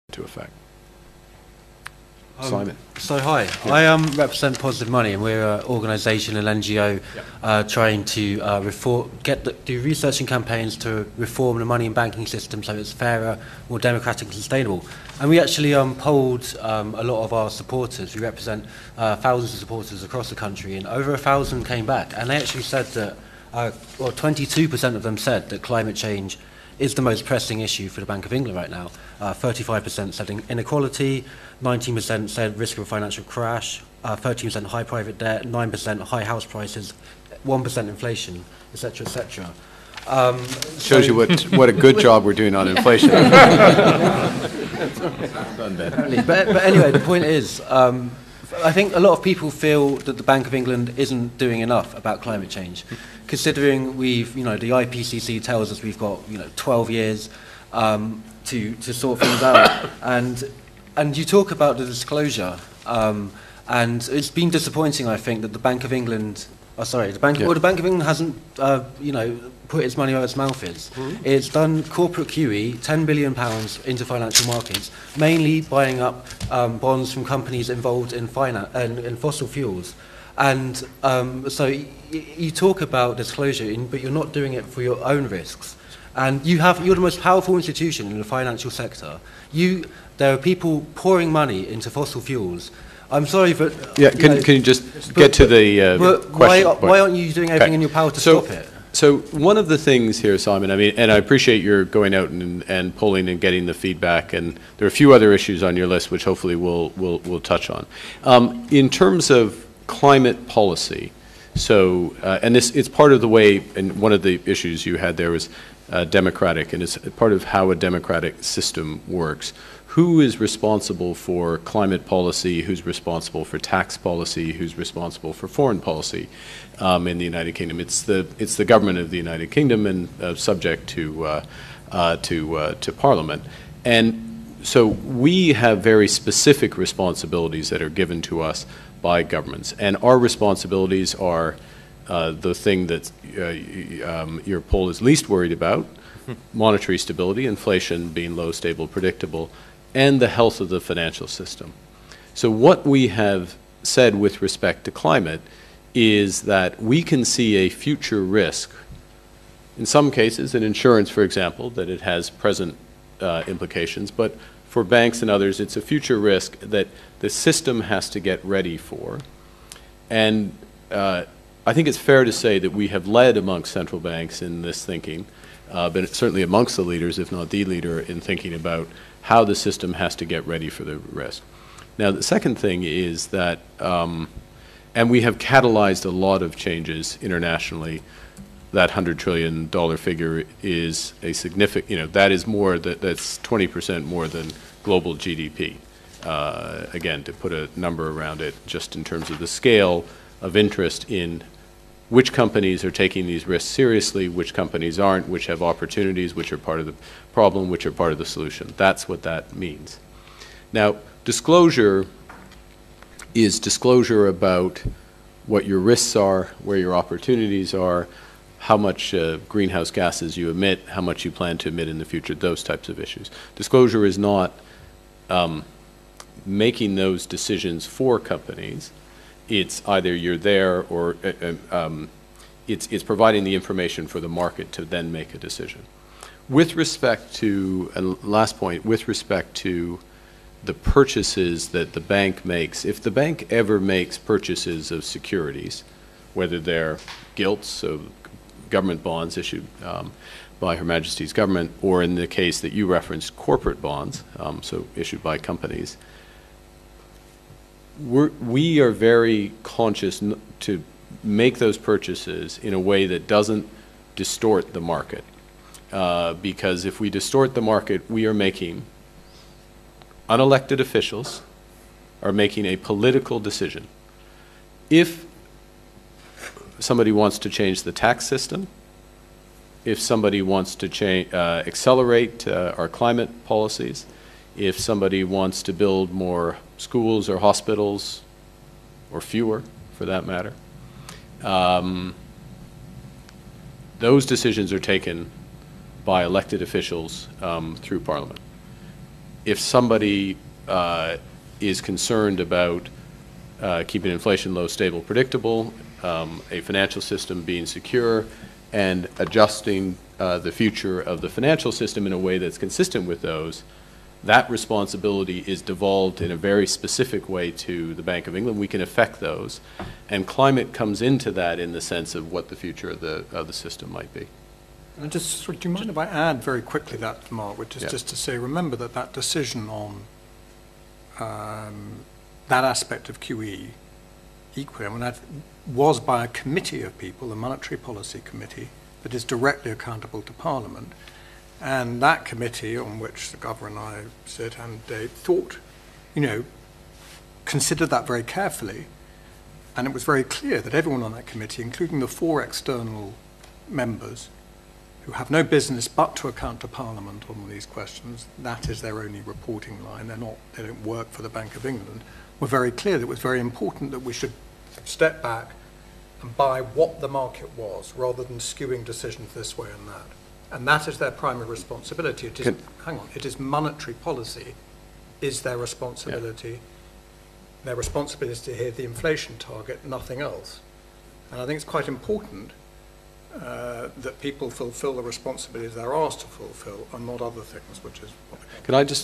Positive Money questions Mark Carney on climate
Simon from Positive Money asks Bank of England Governor Mark Carney about the Bank's failure to act on climate change, at its Future Forum, 28 January 2019.